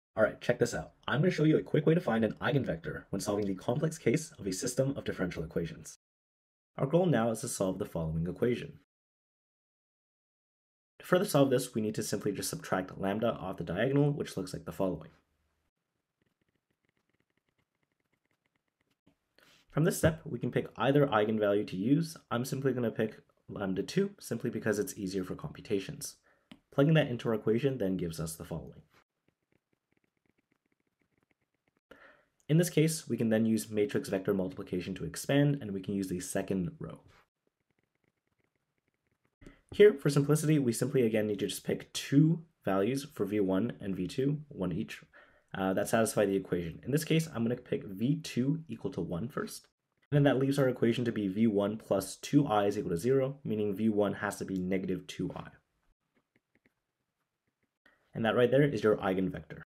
Shortcut for finding an eigenvector with complex eigenvalues
In this short, I go over a shortcut for finding an eigenvector for a matrix with complex eigenvalues. This is extremely useful for the complex case of system of differential equations.